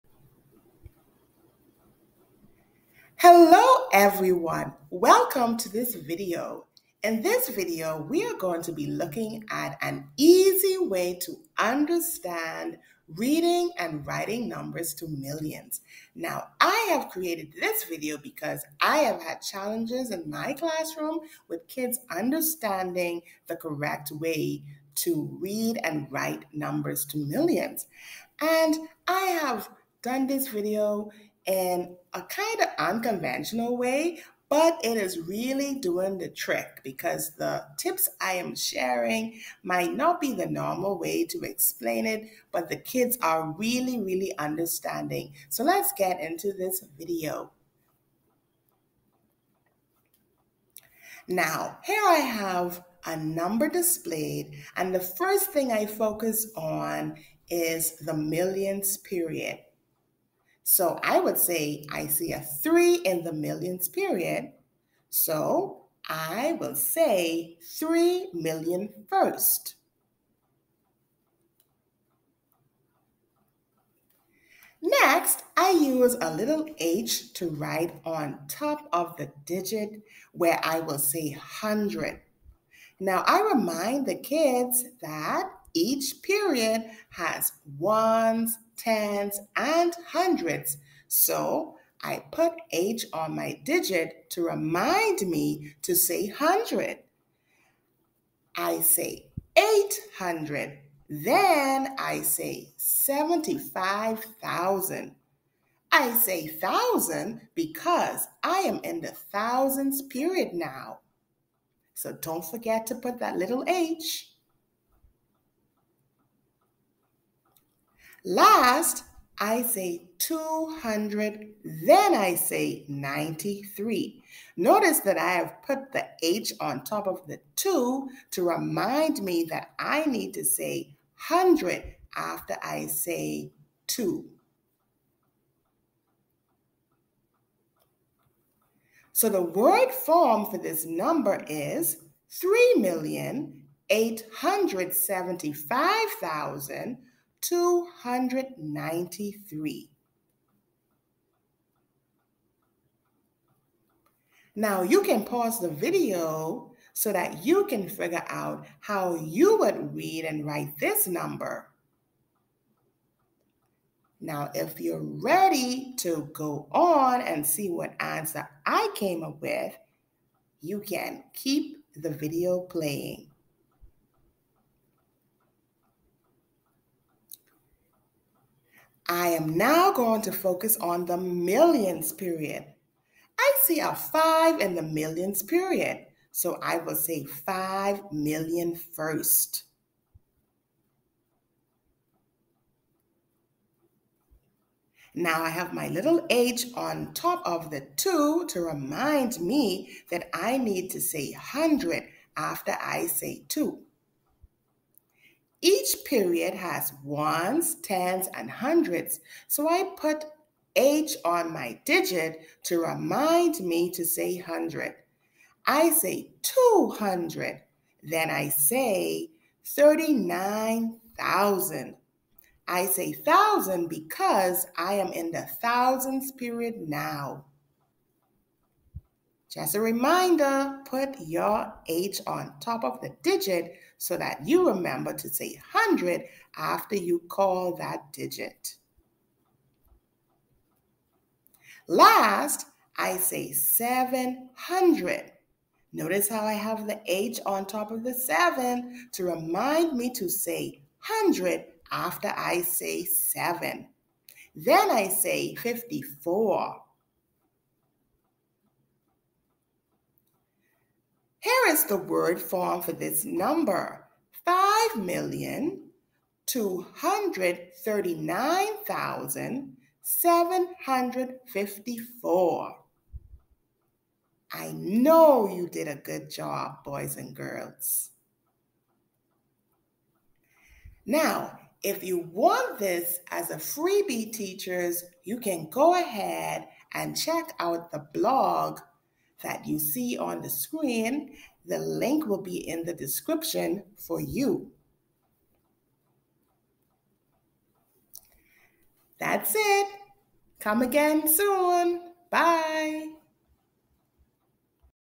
Reading and Writing Numbers to Millions (Step by Step Reading and Writing Numbers to Millions )
Reading and Writing Numbers to Millions can be a challenge. I have had some challenges with some of my kids understand how to do it so I came up with this unconventional way which I feel is a simple step by step way to understand how to read and write numbers to millions.  Get these as Google slides for free by click on the blog link: